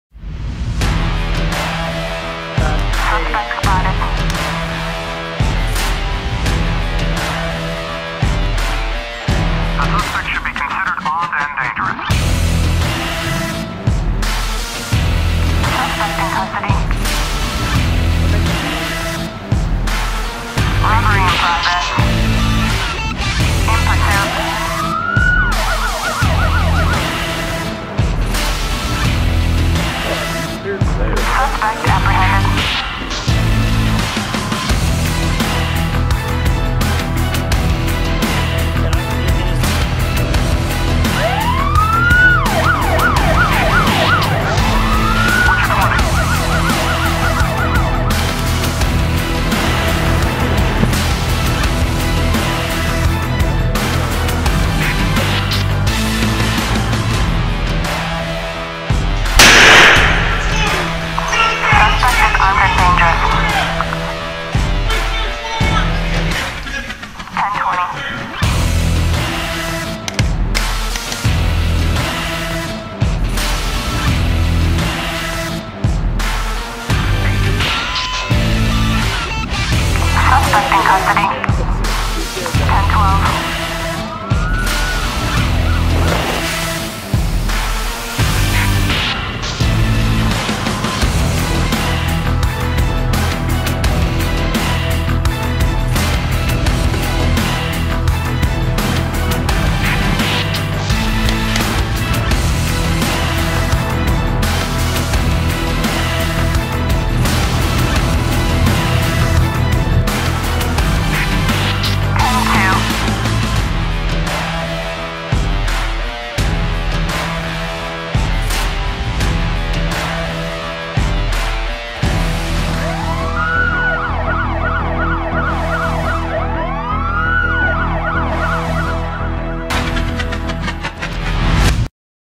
Cape Girardeau Police Recruitment Video
Thank you Randy McWilson at the Cape Girardeau Career and Technology Center for our recruitment video. for PD positions. for more information. Watch this video for more about training at Cape PD